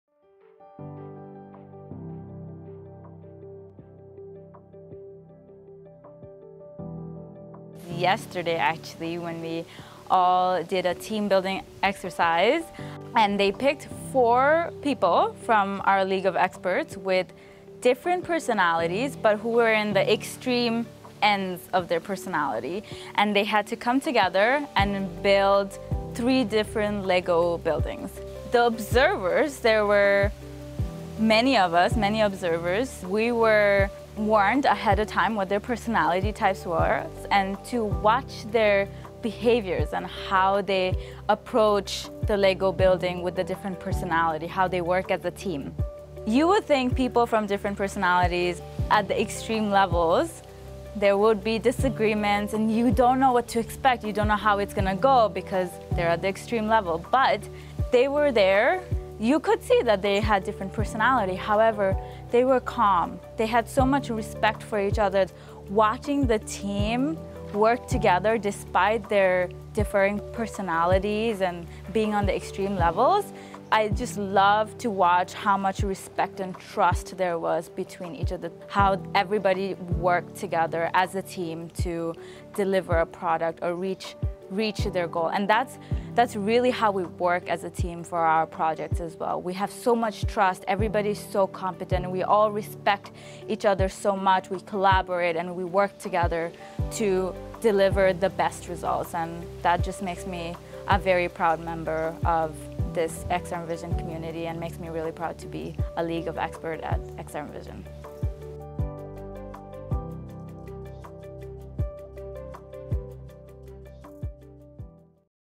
League of Experts / Tribu d'experts | Leoza Kabir Barker
In this first installment of our "League of Experts" series, our functional analyst, Leoza Kabir Barker, shares with us a proud moment in our league of experts. She recounts a recent experience in which four colleagues, each with distinct personalities according to the Kolbe A test, collaborated harmoniously despite their differences. It's a perfect example of how we work at XRM Vision: together, in diversity, to achieve excellence.

Dans cette première capsule de notre série "Tribu d'experts", notre analyste fonctionnelle, Leoza Kabir Barker, partage avec nous un moment de fierté au sein de notre tribu d'experts. Elle raconte une expérience récente où quatre collègues, chacun avec des personnalités distinctes selon le test Kolbe A, ont collaboré harmonieusement malgré leurs différences. C'est un exemple parfait de la façon dont nous travaillons chez XRM Vision : ensemble, dans la diversité, pour atteindre l'excellence. Ne manquez pas cette histoire inspirante et restez à l'écoute pour plus de récits captivants de notre tribu d'experts.